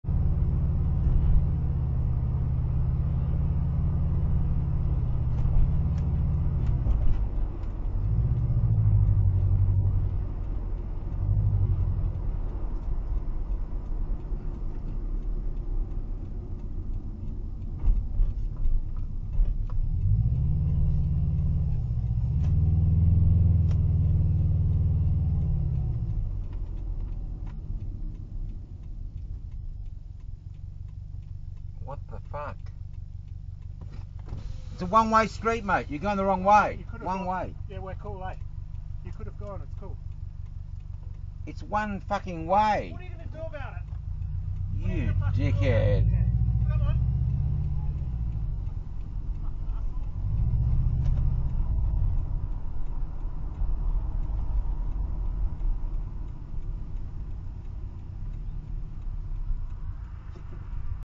Cyclists Tea Tree Gully
Perseverance Rd Tea tree Guuly (N.E Rd cnr)
2 cyclists come head on at me, the wrong way down a One Way Rd
then verbally abuse me, after I stopped for them ?